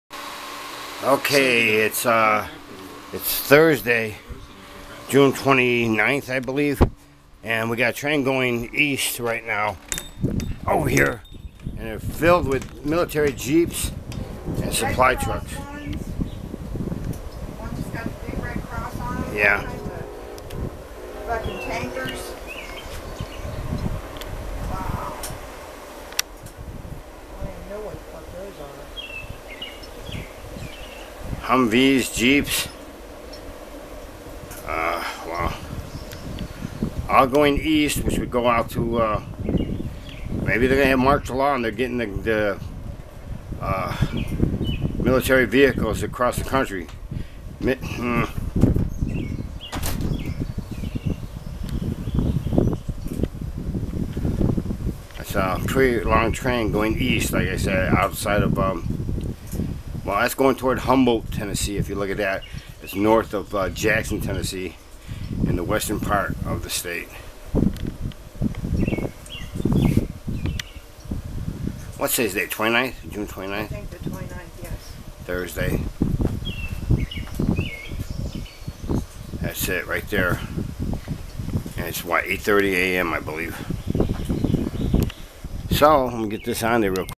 PREPARING FOR.... MARTIAL LAW? CIVIL UNREST?
* MAY CONTAIN STRONG LANGUAGE
Train traveling EASTWARD, outside HUMBOLDT. TN loaded with military vehicles. What could they POSSIBLE be for? Word is this  "SEPTEMBER/FALL"  CIVIL UNREST  MAY  EXPLODE..  Get your family  READY... JUST IN CASE. There's  ALSO  talk of  PRESIDENT  TRUMP  BEING "ASSASSINATED?  W.T.F.?